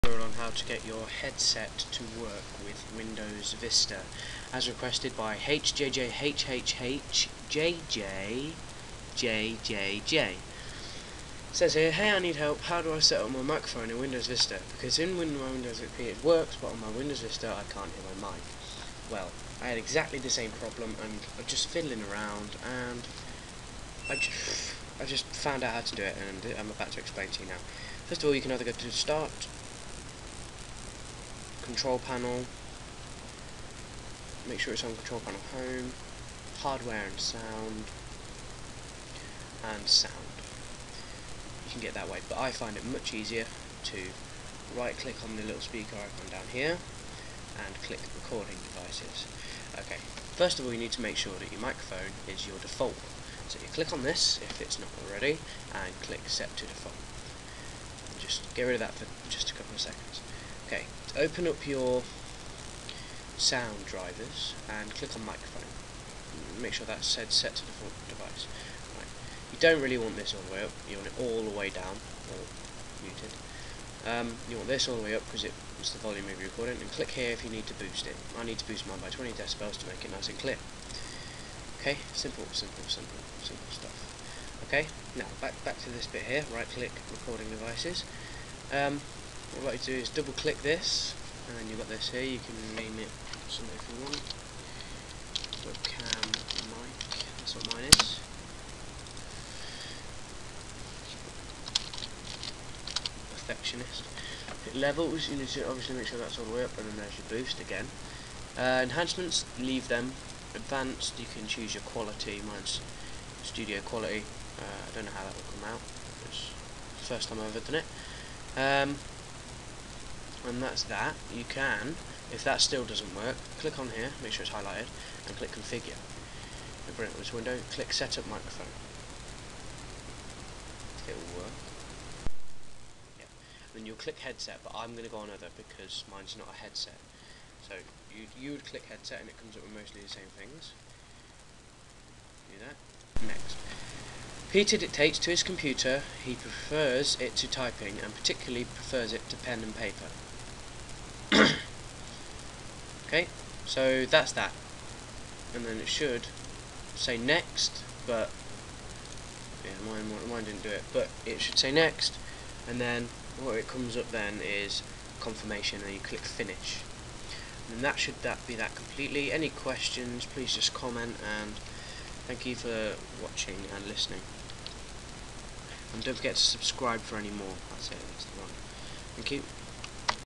How To Get Your Headset To Work In******* Windows Vista*******
this tutorial was posted as of a request... it is basically just to tell you how to configure your headset with your pc, or just about any other microphones .

EDIT: i dont know how to use XP sorry, so dont ask on XP

help easy microphone xp vista windows 2008 recording microphone volume